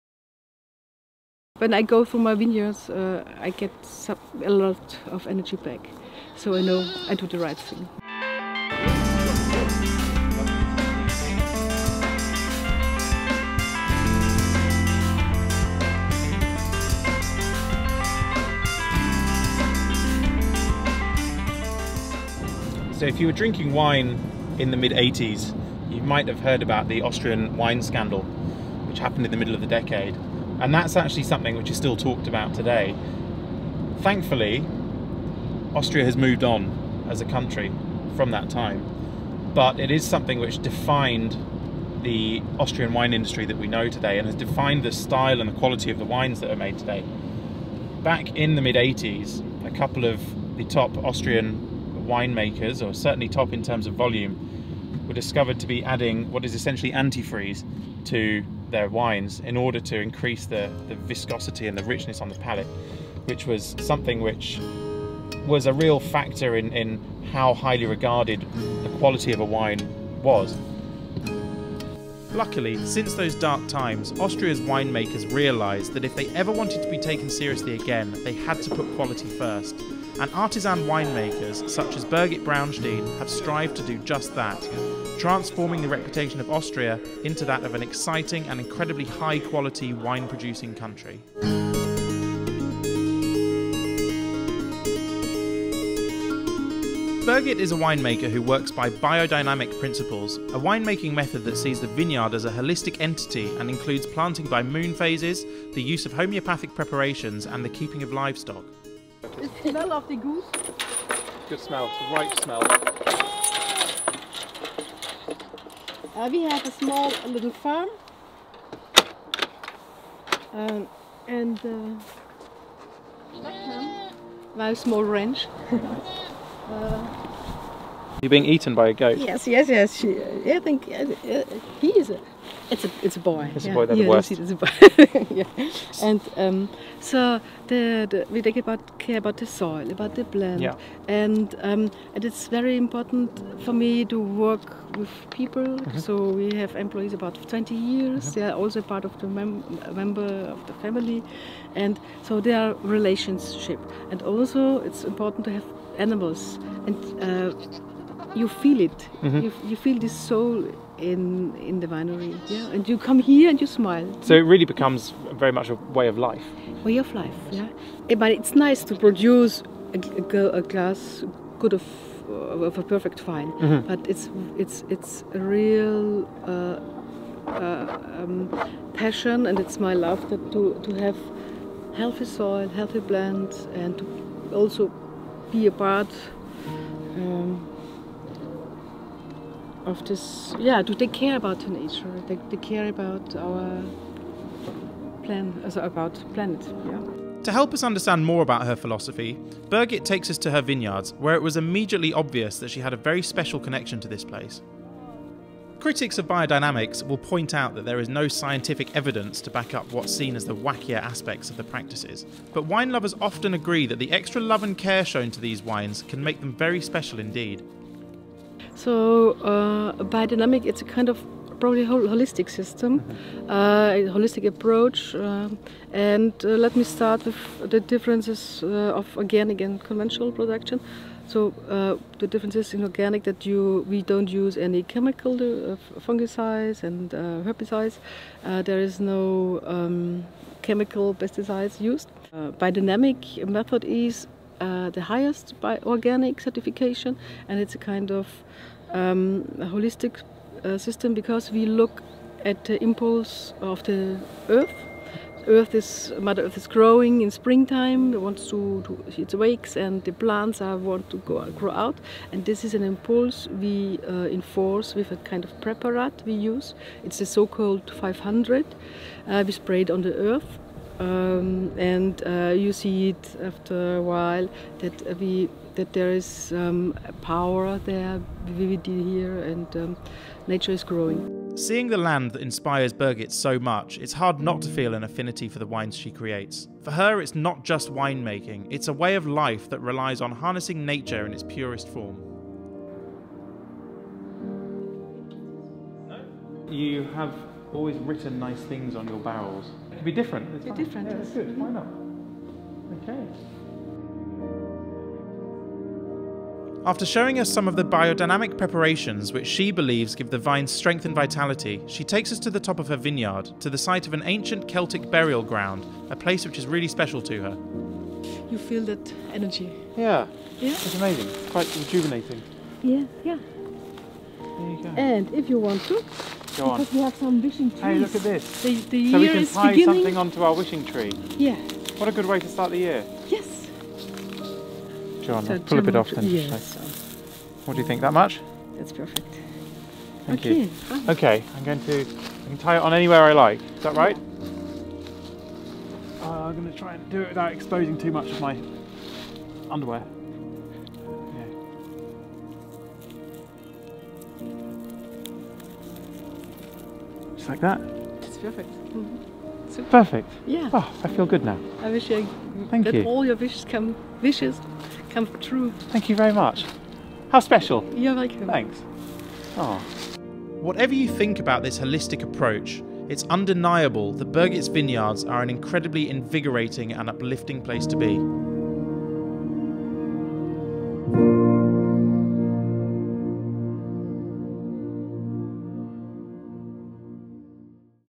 Artists of Austria Episode 3, Birgit Braunstein: A Holistic Approach
Wine Society buyer, Freddy Bulmer, experiences more than just great wine when he meets biodynamic proponent Birgit Braunstein, finding an ancient burial ground and ley lines are part of the story of this fascinating winery.

to see the whole story and to buy the wine.